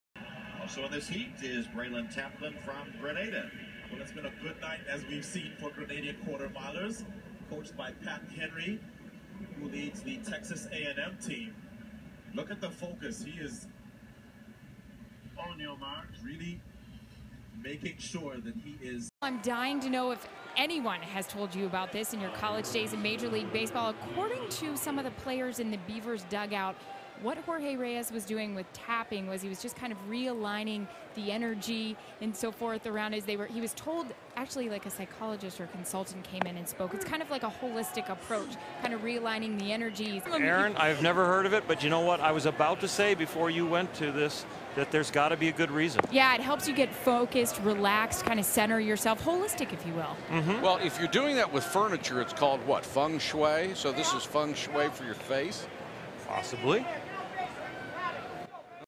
Atleta olímpico e jogador de beisebol usam a EFT para melhorar desempenho
Bralon Taplin velocista de Granada, Olimpiada Rio 2016 e Jorge Reyes, campeão como jogador de beisebol dos EUA, são flagrados usando a EFT para melhorar o desempenho em competição.